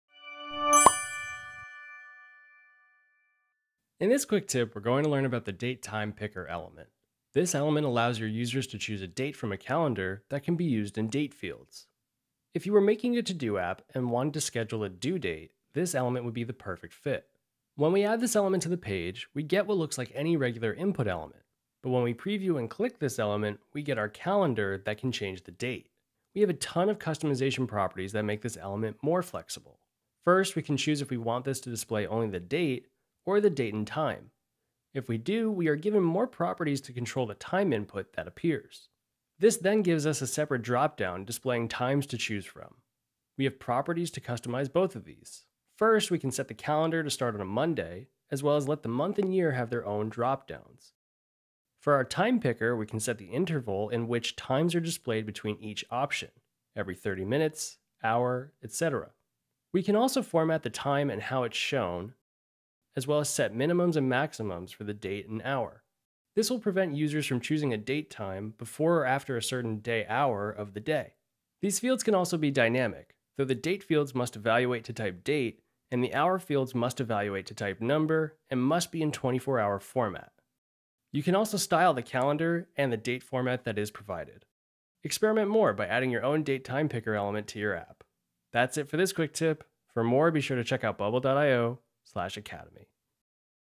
How to Use The Date Time Picker Element | Bubble Quick Tip
0:00 - Intro
0:06 - Date/Time Picker Overview
0:26 -  Date/Time Picker Properties
0:30 - Date vs Date/Time
1:20 - Styling the Date/Time Picker
1:28 - Outro